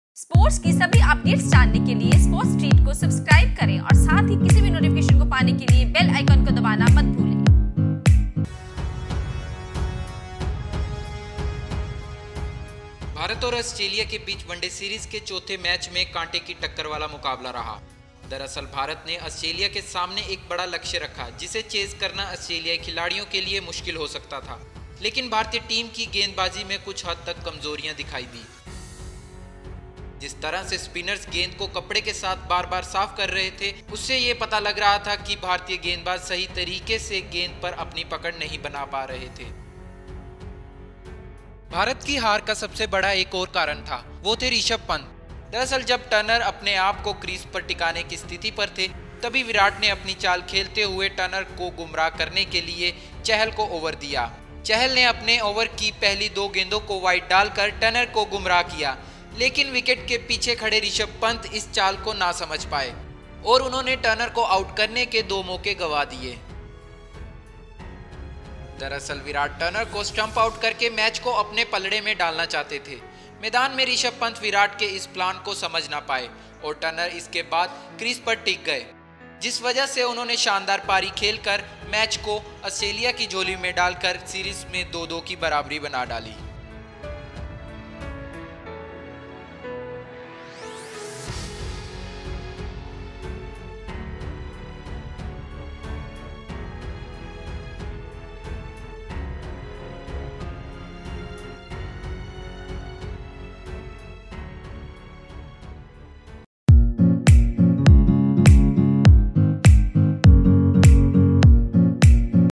INDvsAUS: जानिए मोहाली में भारत के हारने की असली वजह क्या थी
भारत और अस्ट्रेलिया के बिच वनडे सीरीज के चौथे मैच में कांट की टक्करर वाला मुकाबला रहा। दरअसल भारत ने अस्ट्रेलिया के सामने एक बडा Target रखा जिसे चेस करना अस्ट्रेलियाई खिलाडियों के लिए मुश्किल हो सकता था। लेकिन भारतिय टिम की गेंदबाजी में कुछ हद तक कमजोरी दिखाई दी।